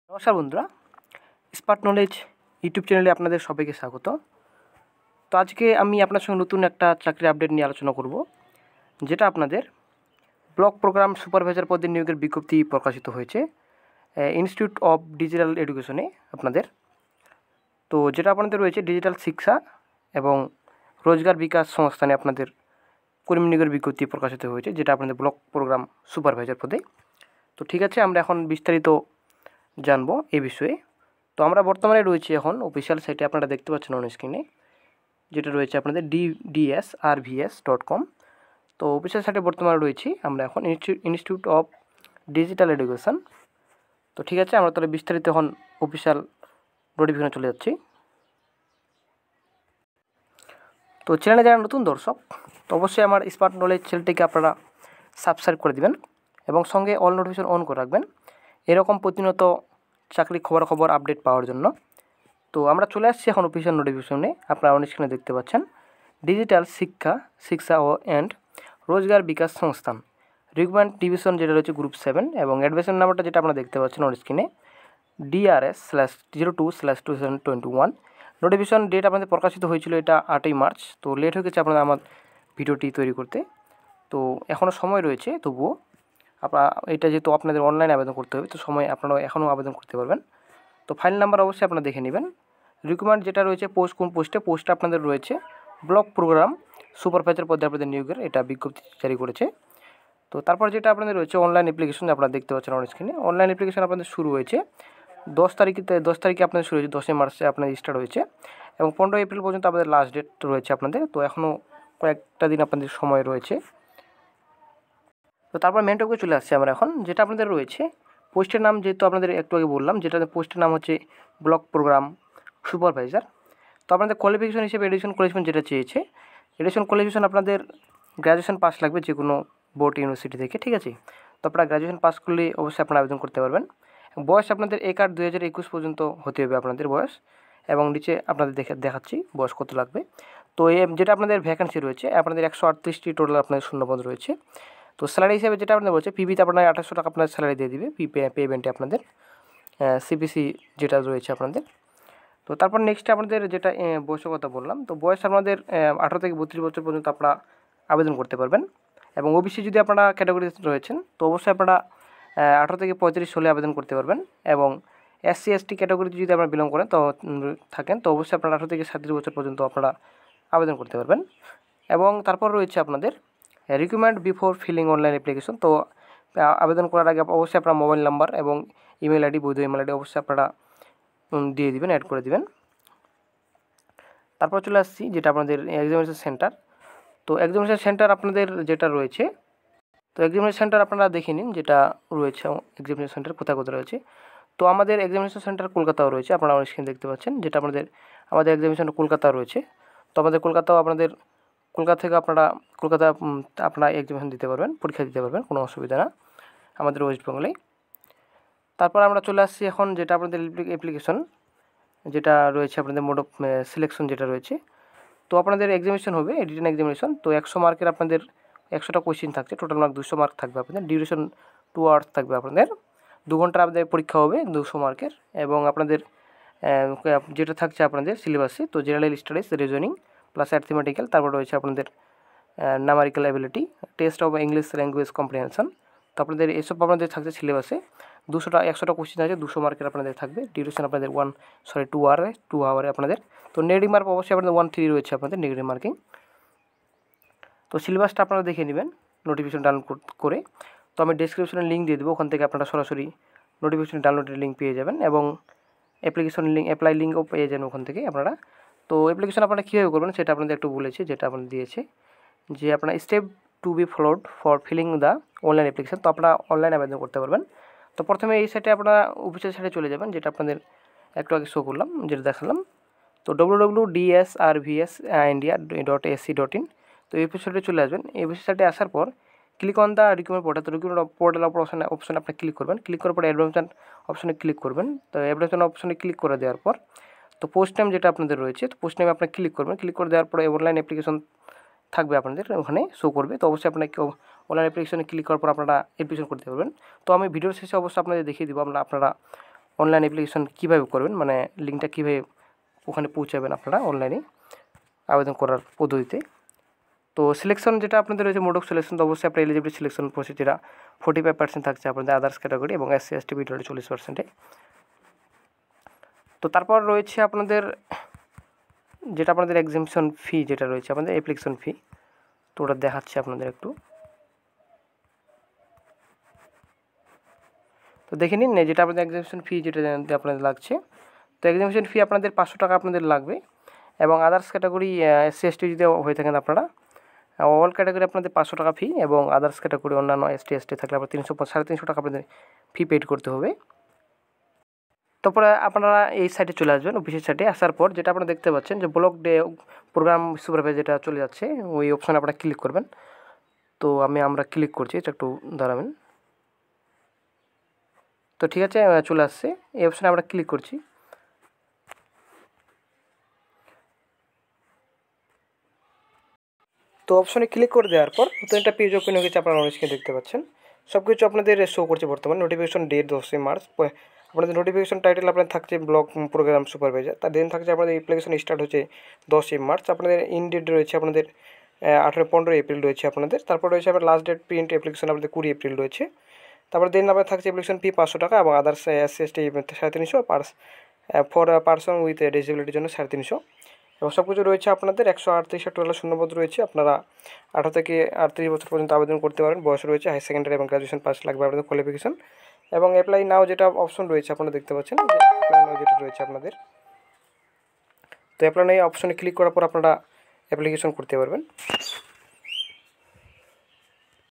❇️শিক্ষা এবং রোজগার বিকাশ সংস্থান-এ কর্মী নিয়োগের বিজ্ঞপ্তি👇👇
❇️BHEL এ কর্মী নিয়োগ--

🌿আবগারি কনস্টেবল Main Exm ডেট প্রকাশিত/WB PSC ফুড এস. আই(FOOD S.I) নিয়োগ-2021(যোগ্যতা/বয়স সহ বিস্তারিত👇👇)

❇️ Official Notification--(নিচে👇👇)
❇️অস্টম পাশে রেল টিকিট কালেক্টর(T.C)👇

❇️GOOD NEWS/BAD NEWS-🌿ফুড এস.আই -নতুন বিজ্ঞপ্তি/ক্লার্কশিপ পার্ট-2 ফল/আর্মি র‍্যালি আপডেট/ICDS ফল কবে/আট বাহিনী কনস্টেবল-80হাজার👇👇

❇️Rpf কবে বিজ্ঞপ্তি বিস্তারিত